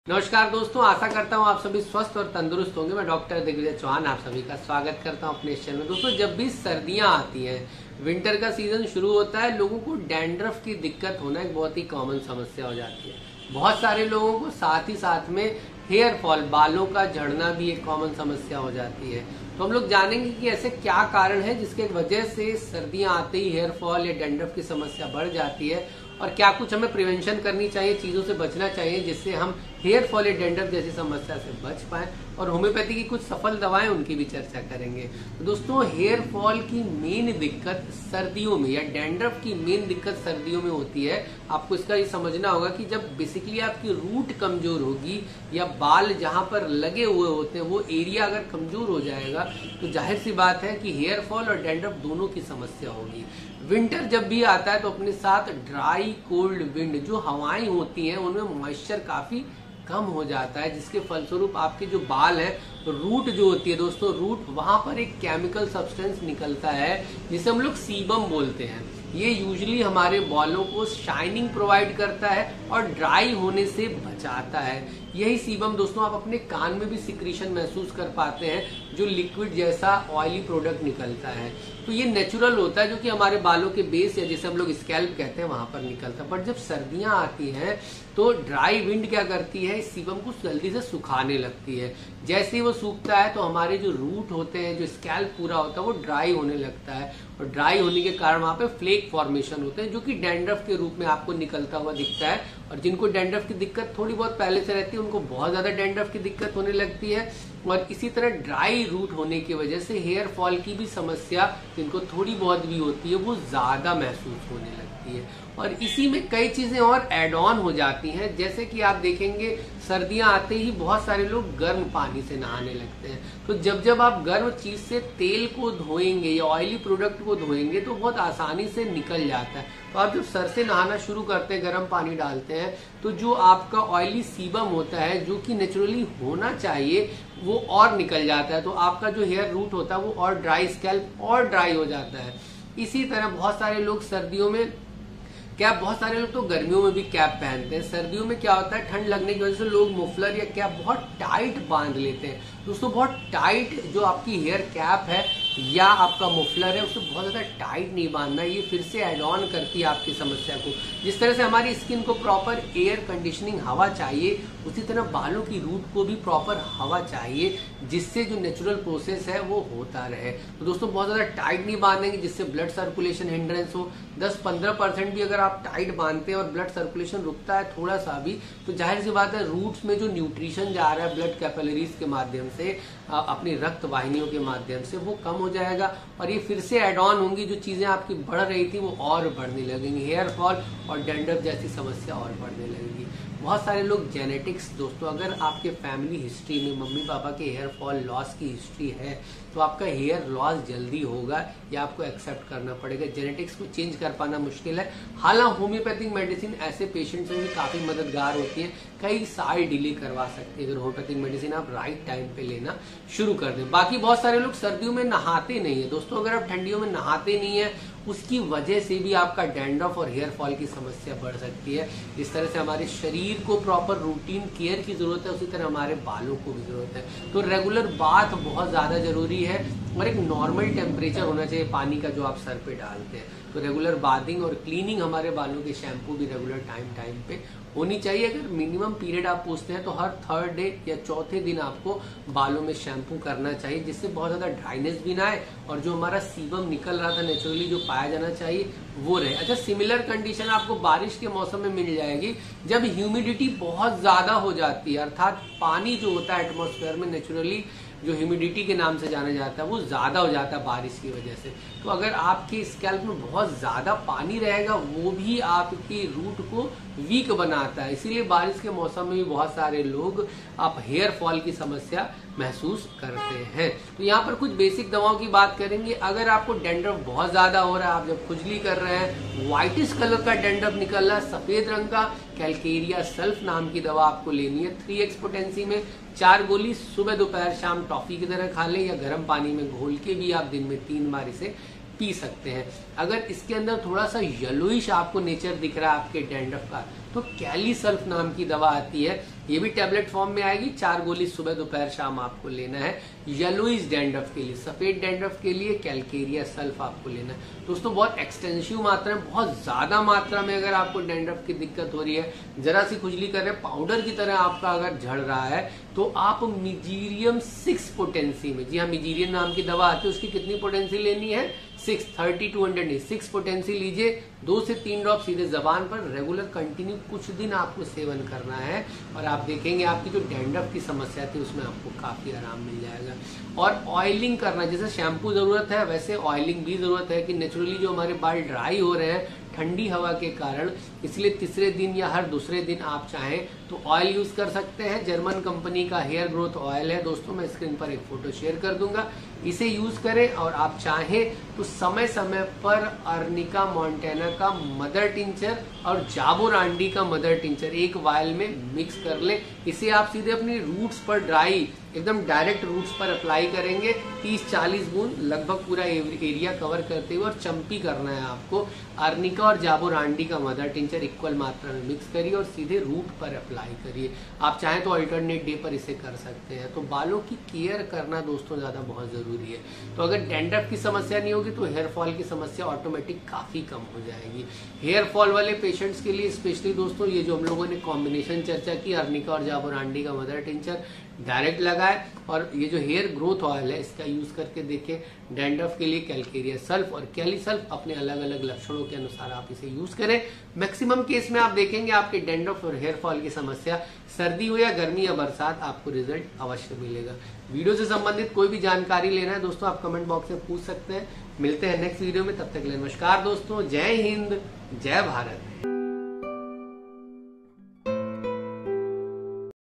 Dandruff and Hair fall in winter!!सर्दियों में डेंड्रफ और बालों का झड़ना
Namaskar Doston,
                                   Is video mai batae gae homeopathic medicine aap bahot se problem mai sath de sakti hai.
Agar aapko sardiyon mai dandruff aur achanak se baal jhadne jaise dikkat hoti hai to video mai batae gae davaon ko lakshno k anusaar le aapko 100% result milega.
Agar aap paramarsh lena chahte hai to hmare whats app number par message kar appointment lelen aur deye gae time par call kare .
Video agar aacha laga ho to like aur share jarur karen.

Dhanyawad.
Regards-Dr Digvijay chauhan